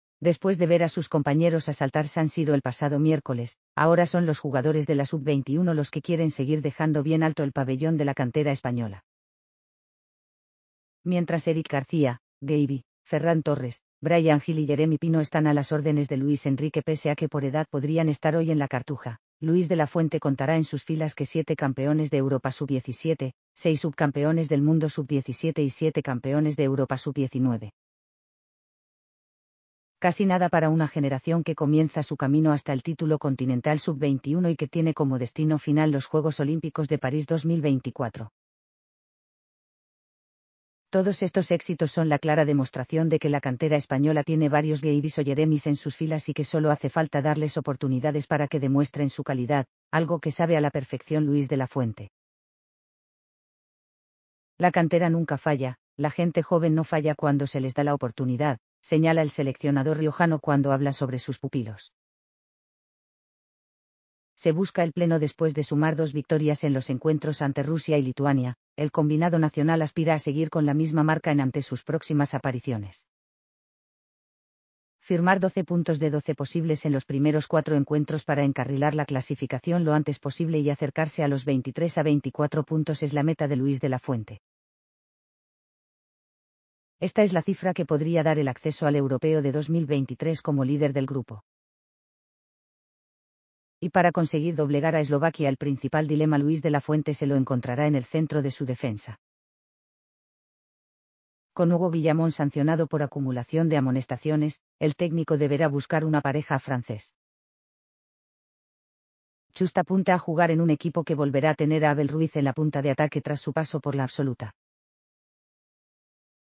España - Eslovaquia: Los futuros 'Gavis'
Después de ver a sus compañeros asaltar San Siro el pasado miércoles, ahora son los jugadores de la sub 21 los que quieren seguir dejando bien alto el pabellón de la cantera española.  Mientras Eric García, Gavi, Ferran Torres, Bryan Gil y Yeremy Pino están a las órdenes de Luis Enrique pese a que por edad podrían estar hoy en La Cartuja, Luis de la Fuente contará en sus filas que siete campeones de Europa sub 17, seis subcampeones del mundo sub 17 y siete campeones de Europa sub 19.  Casi nada para una generación que comienza su camino hasta el título continental sub 21 y que tiene como destino final los Juegos Olímpicos de París 2024.   Todos estos éxitos son la clara demostración de que la cantera española tiene varios Gavis o Yeremys en sus filas y que sólo hace falta darles oportunidades para que demuestren su calidad, algo que sabe a la perfección Luis de la Fuente.  "La cantera nunca falla, la gente joven no falla cuando se les da la oportunidad", señala el seleccionador riojano cuando habla sobre sus pupilos. Se busca el pleno Después de sumar dos victorias en los encuentros ante Rusia y Lituania, el combinado nacional aspira a seguir con la misma marca en ante sus próximas apariciones.  Firmar 12 puntos de 12 posibles en los primeros cuatro encuentros para encarrilar la clasificación lo antes posible y acercarse a los 23-24 puntos es la meta de Luis de la Fuente.  Esta es la cifra que podría dar el acceso al Europeo de 2023 como líder del grupo.   Y para conseguir doblegar a Eslovaquia el principal dilema Luis de la Fuente se lo encontrará en el centro de su defensa.  Con Hugo Guillamón sancionado por acumulación de amonestaciones, el técnico deberá buscar una pareja a Francés.  Chust apunta a jugar en un equipo que volverá a tener a Abel Ruiz en la punta de ataque tras su paso por la Absoluta.